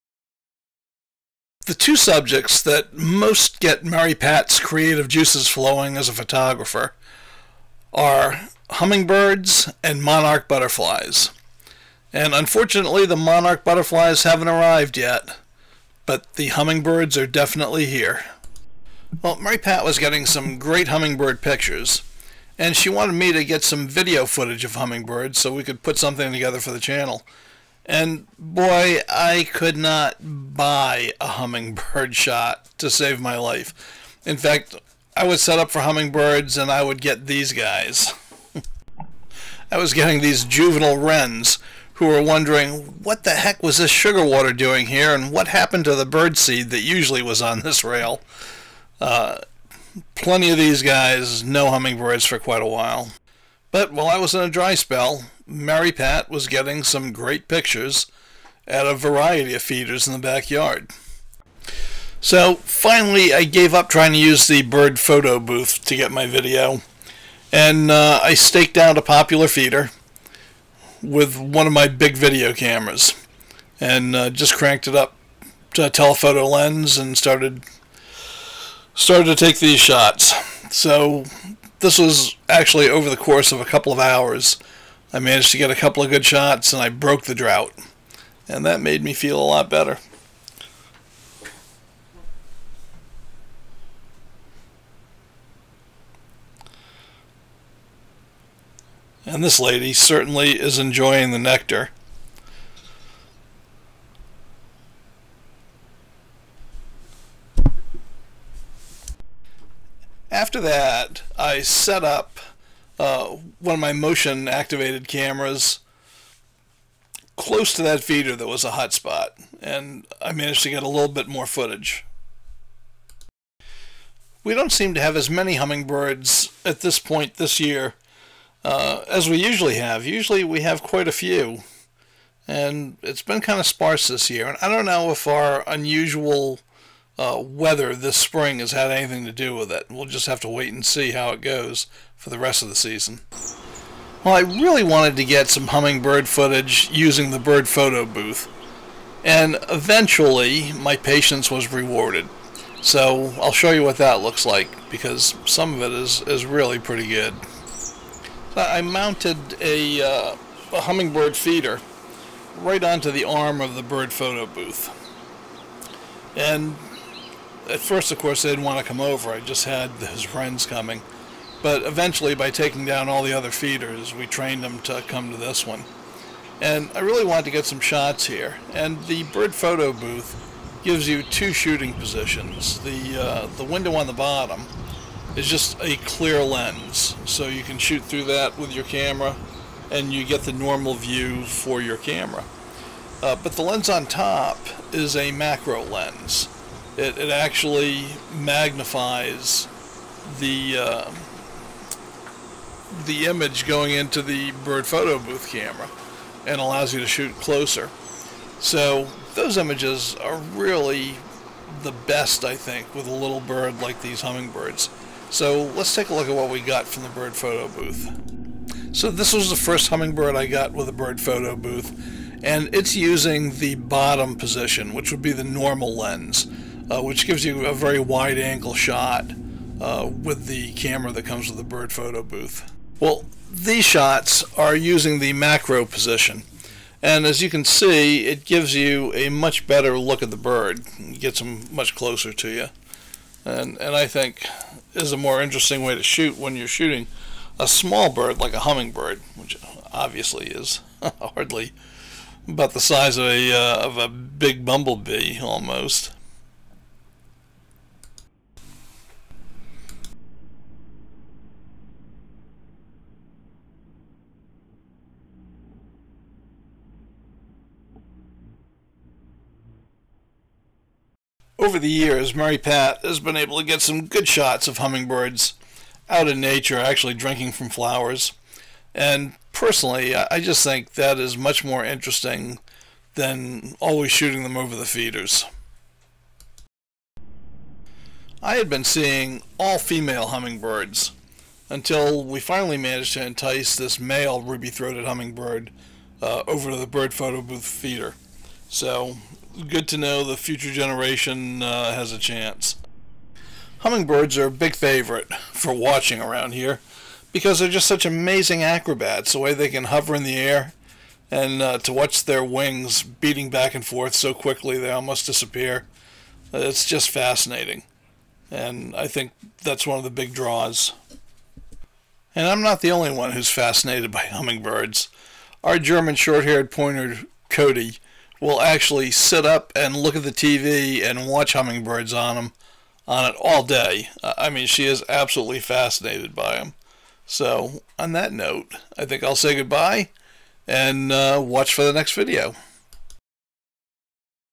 Backyard Hummingbirds
There are two things that gets Mary Pat Rolf’s photographic juices flowing, Monarch butterflies, and Hummingbirds. Unfortunately, the Monarchs are a bit late arriving this year, but, luckily, the Hummers are here!